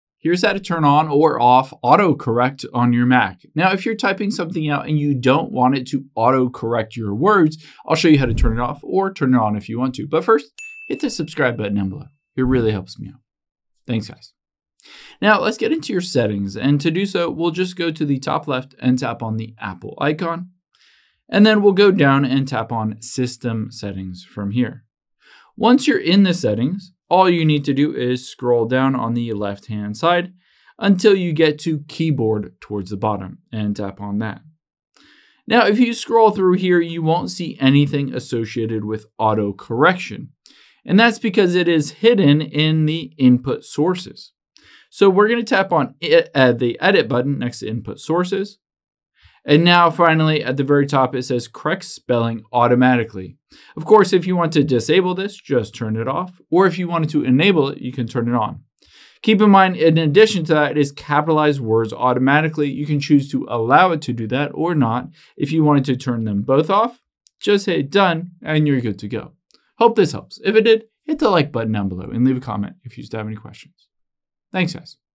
How To Turn On Or Off Auto Correct On Mac
Here's how to enable or disable the keyboard auto correct on your Mac if you find it helpful or annoying.